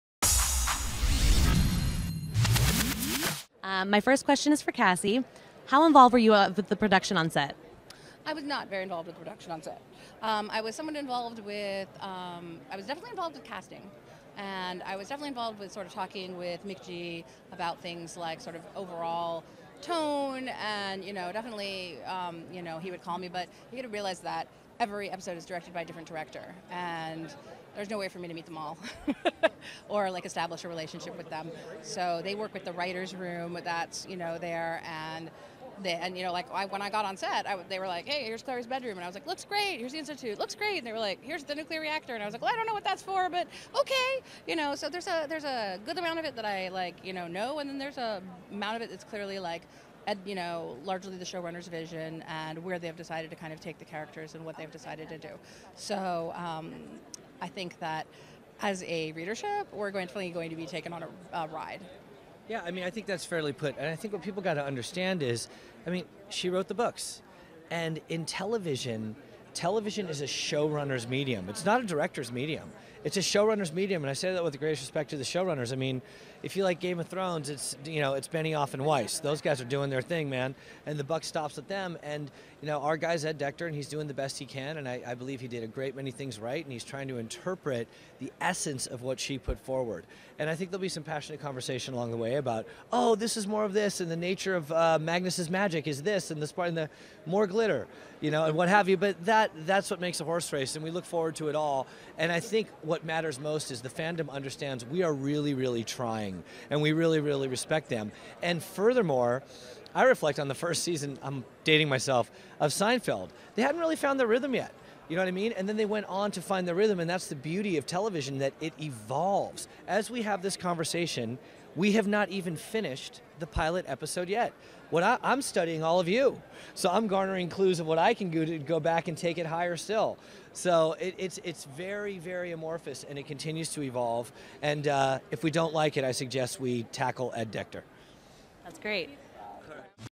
Entevista a Cassandra Clare y a McG para el canal Wikia con subtitulos en español
Subtitulado por: Shadowhunters en español